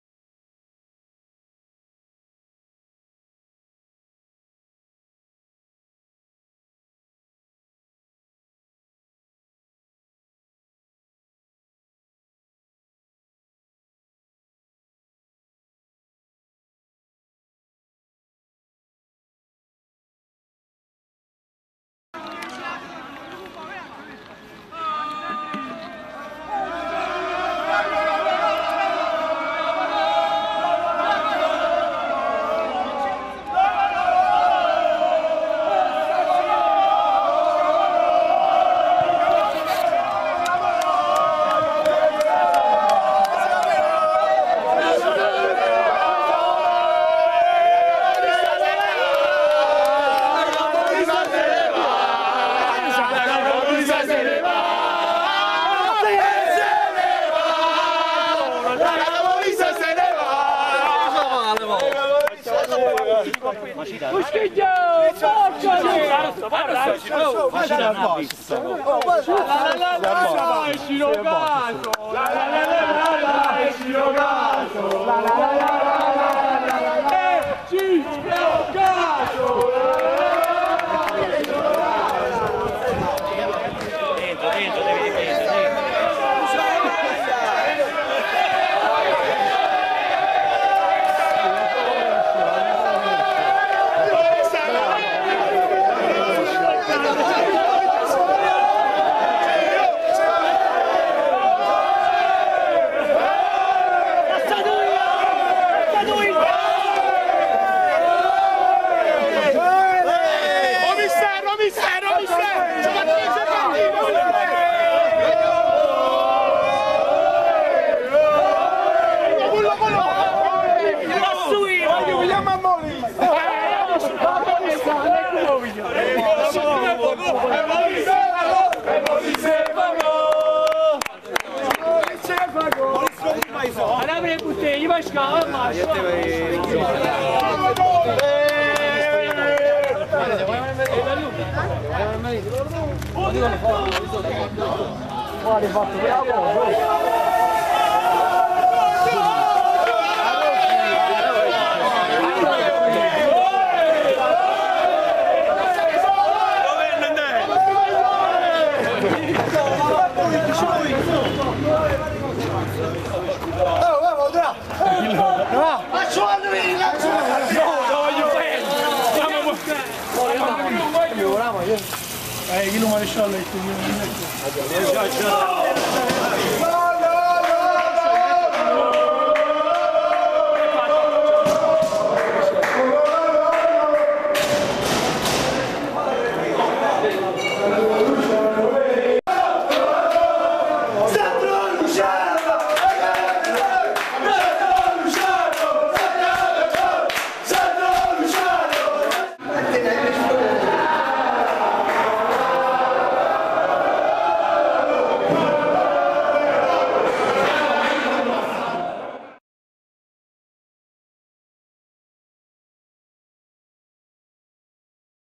Virtus Casollese-Cetara Soccer= 6-2 Festeggiamenti Promozione
1 Maggio 2010 - Immagini gentilmente concesse dall'operatore Giuseppe Petrone. La Virtus Casollese è promossa al Campionato PROMOZIONE.